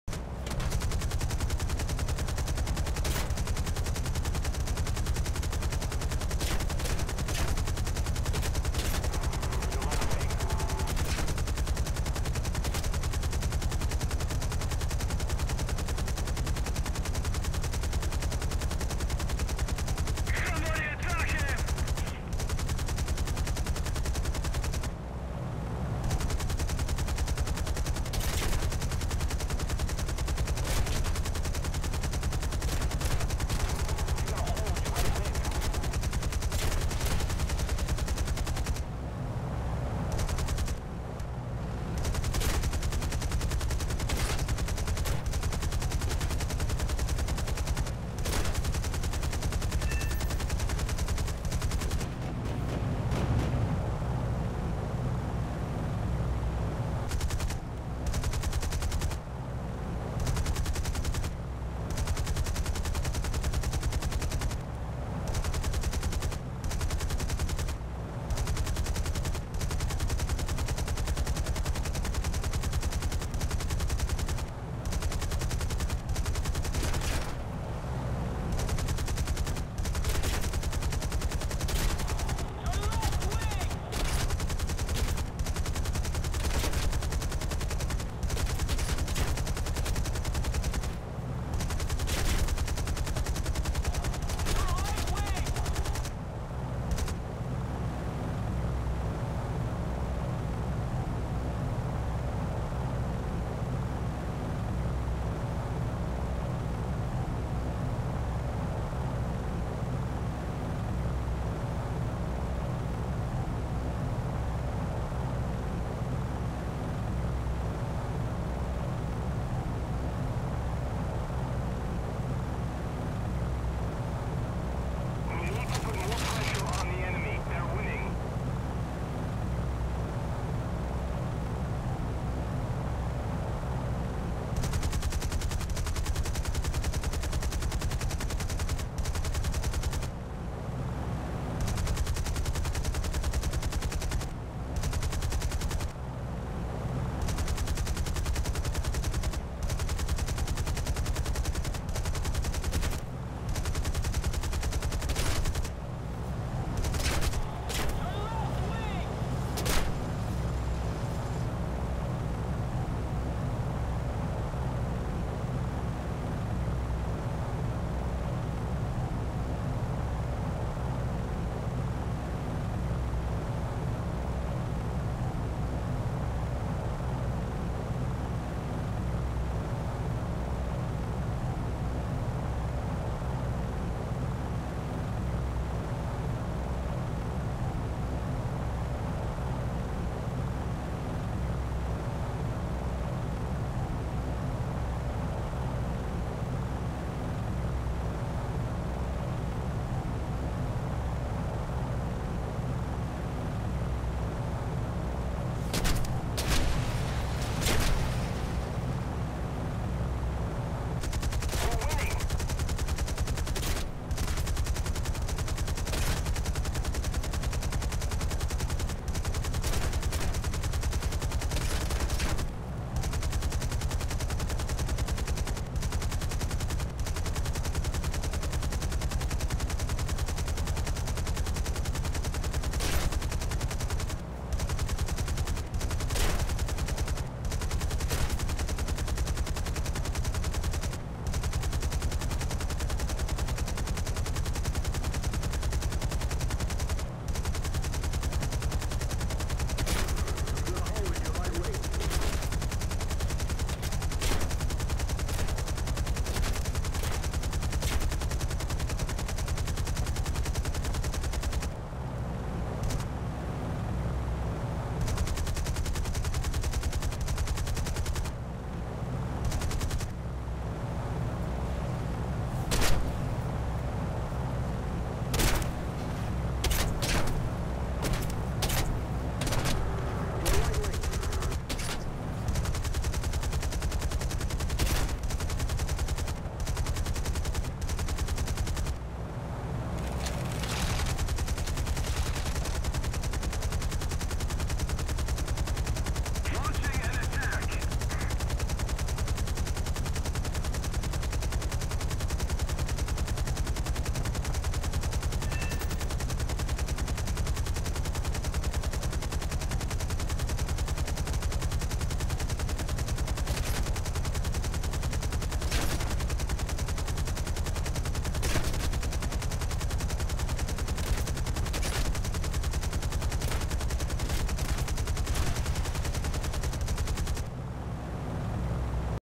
B17 "ACE" maxed gunners
they dont shoot like ACE, poor AI

Note: This is just me watching the AI gunners.